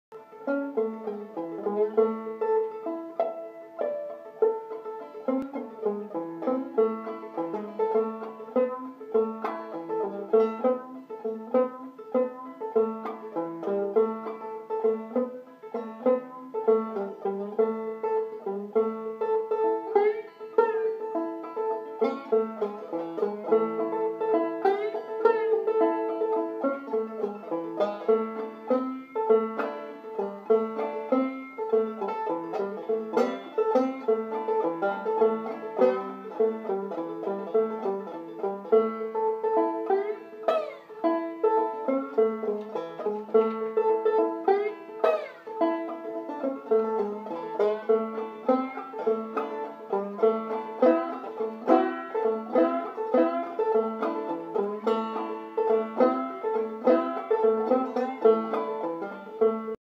Blues on Fretless Banjo
Bruce Ziff plays haunting blues on 1920's banjo as a prelude to Feb 28, 2015 workshop with Leonard Podolak for Winter Roots Festival in Edmonton, Alberta.